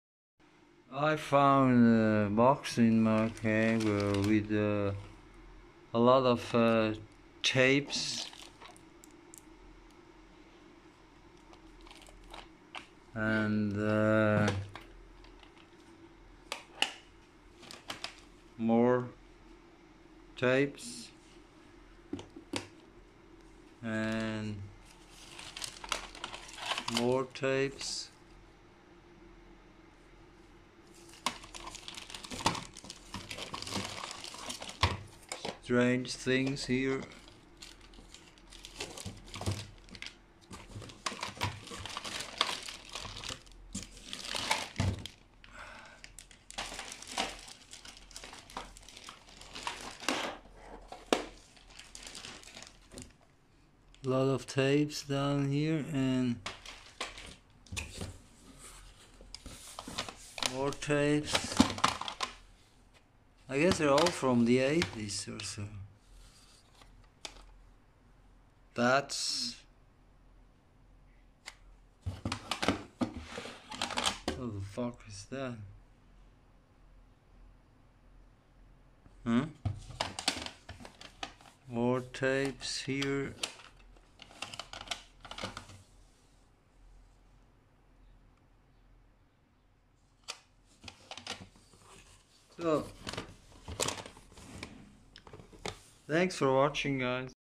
Found a great lot of audio cassettes tapes in the basement
Found a lot of cassettes tapes in the basement ,thmps up if u like,if u don't thumpd down..lol,,